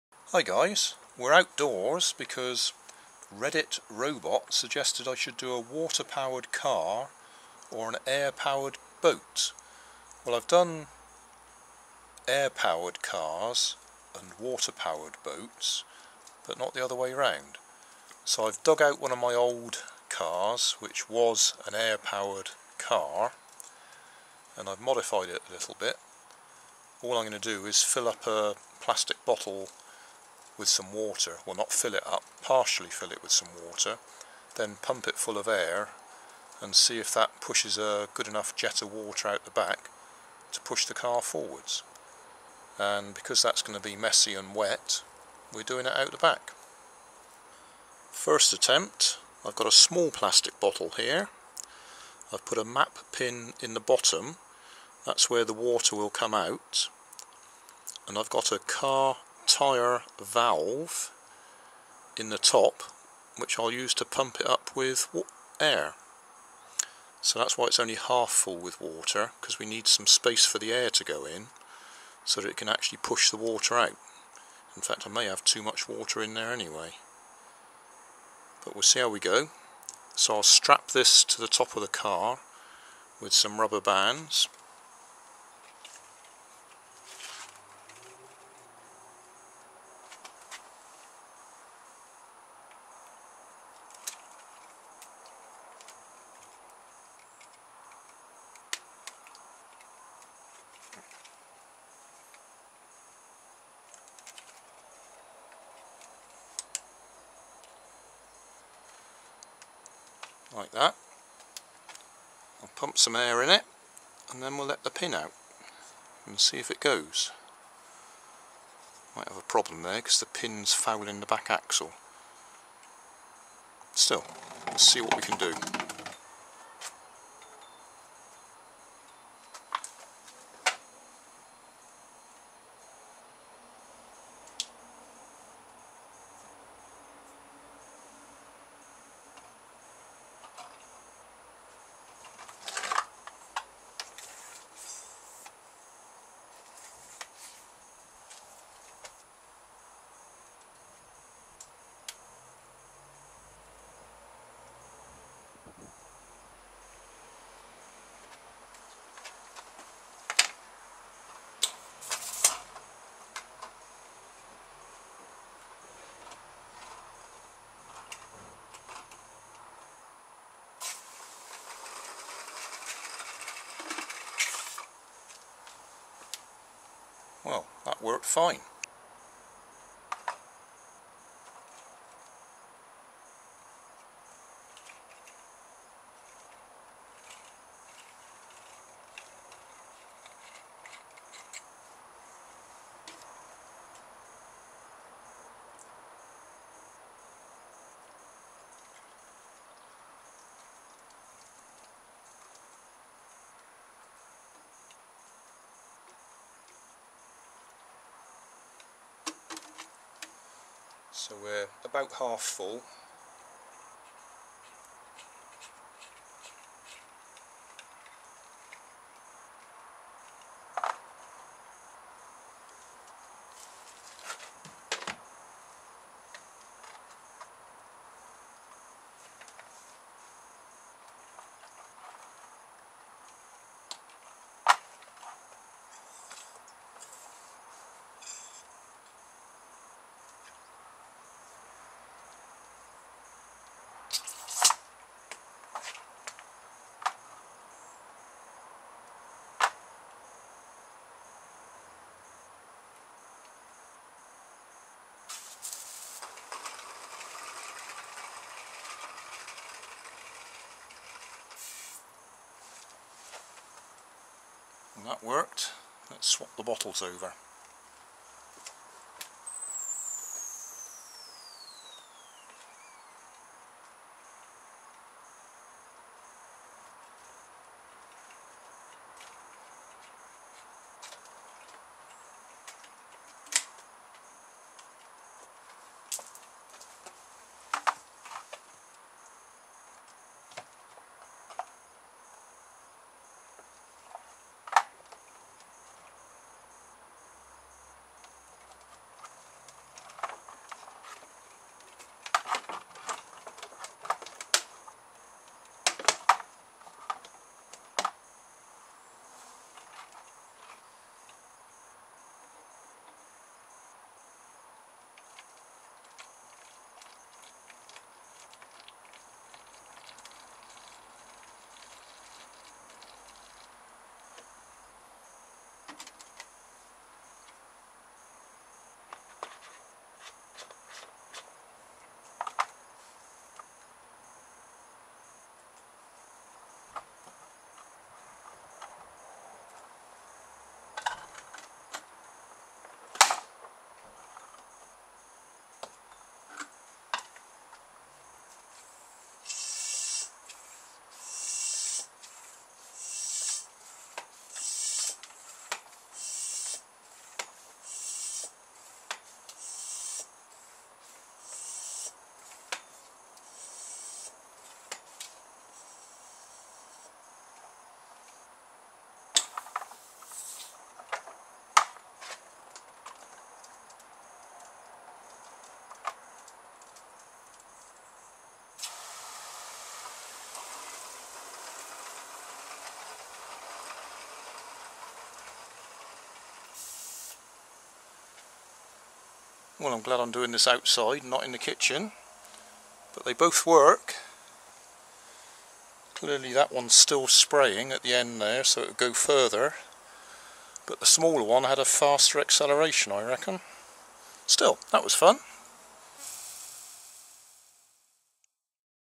Water Jet Car #1
RedditRobot asked "Can you try making a water powered car of some sort or an air powered boat?". I have made air powered cars and water powered boats before but not the other way round so I dug out an old air powered car and modified it.

and this is my old video which includes how to fit the tyre valve in the bottle top